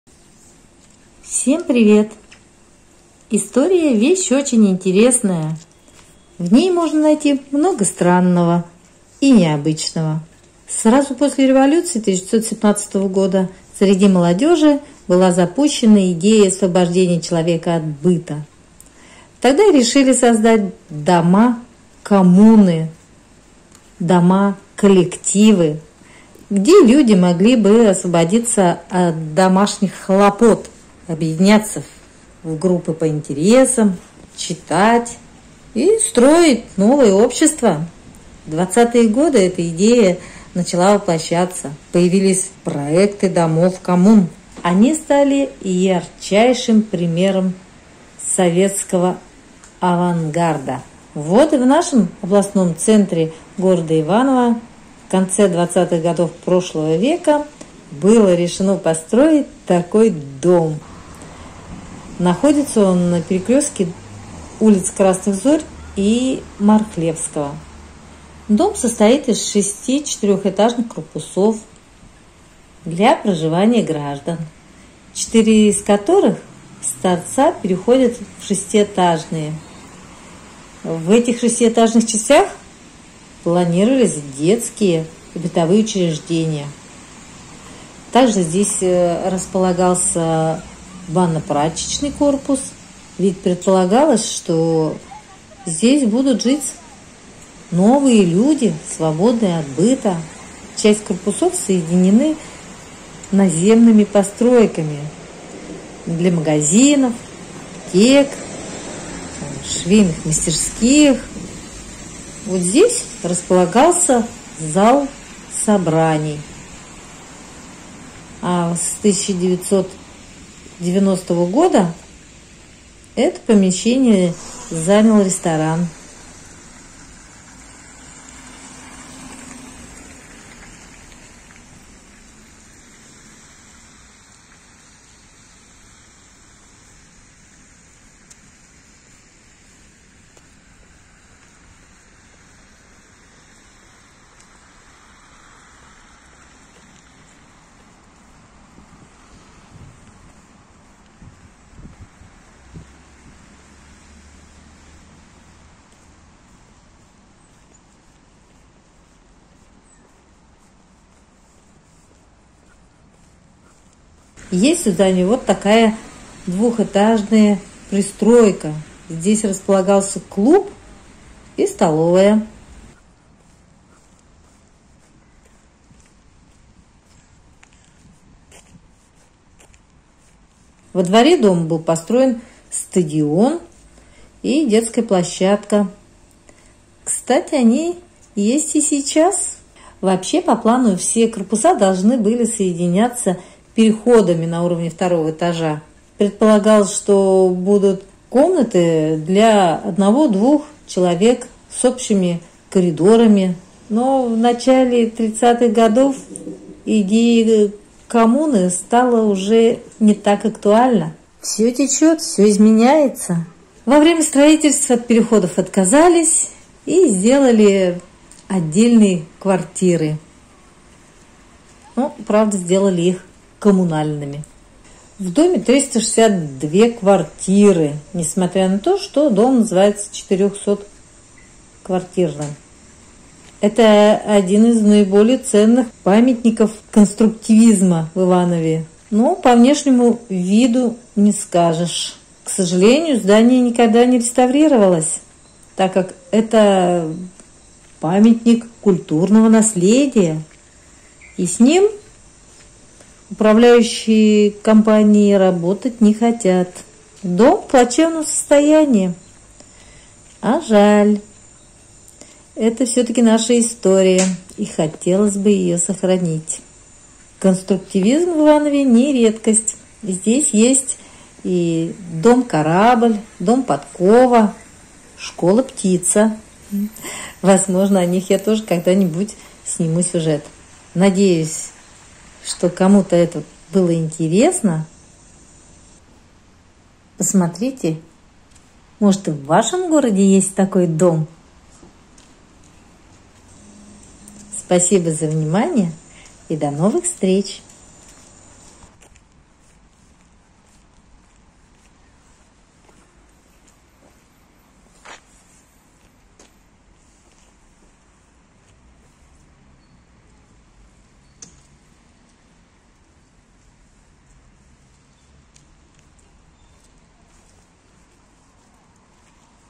Дом коллектива в Иванове, наша история.
Историческое здание в городе Иванове, 400 квартирный дом коммуна, грандиозное сооружение имеет печальный вид.